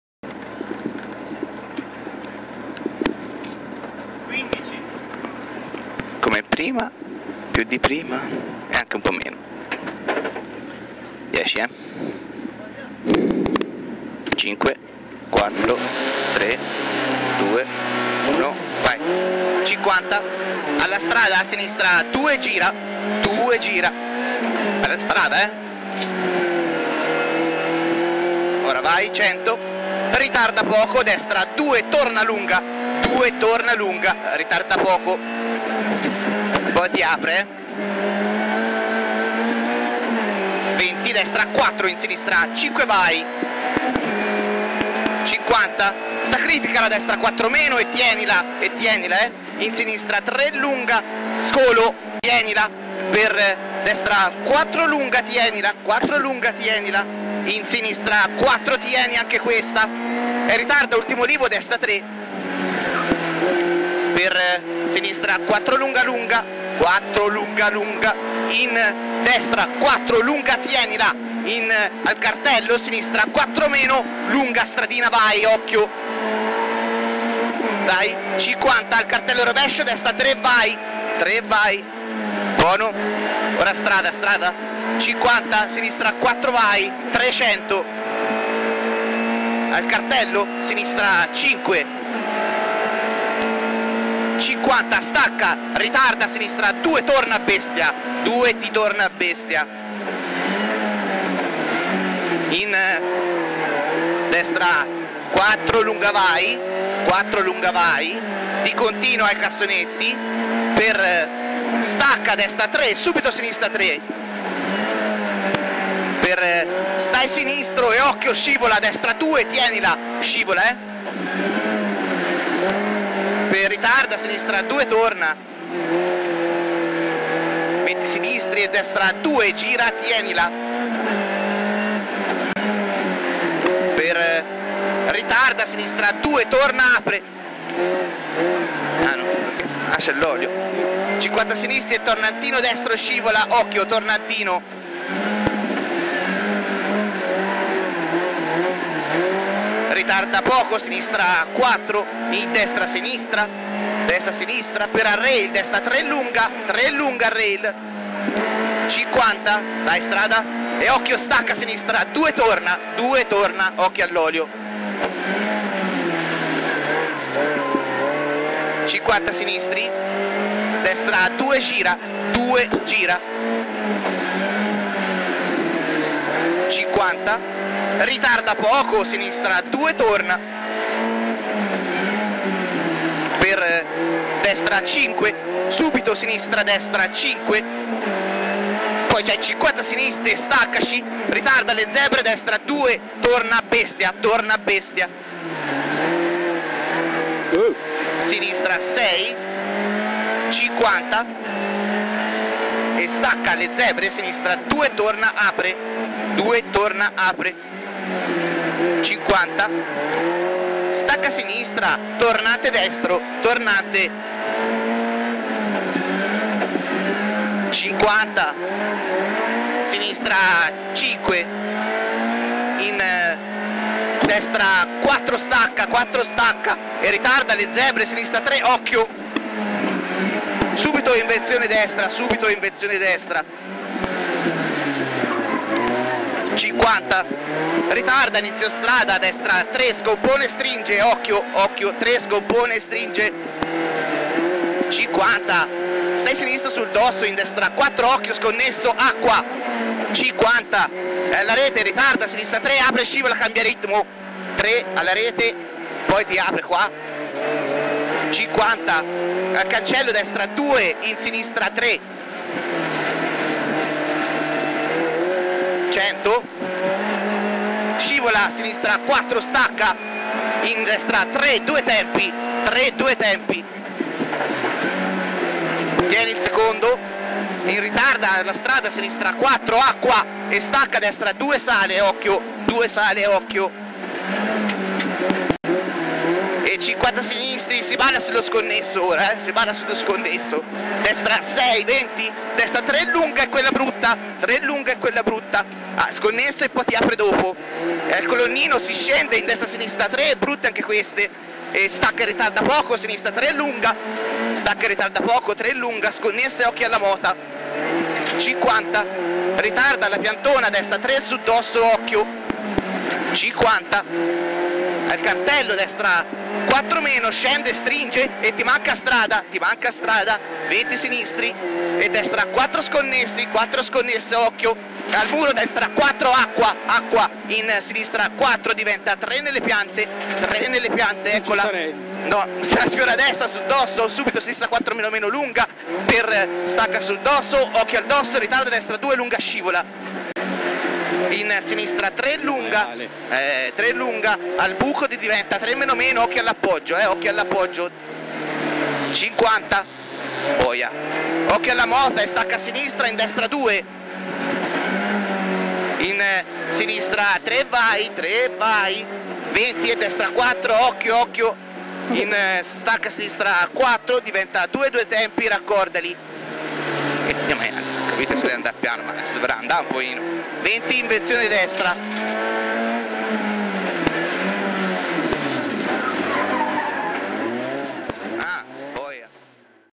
Camera Car Signorini-Marraccini Rally dei Viviai Pistoiesi 2010
Signorini-Marraccini Rally dei Viviai Pistoiesi 2010
Renault CLIO RS N3 Pegaso Team
1° di classe
7° assoluti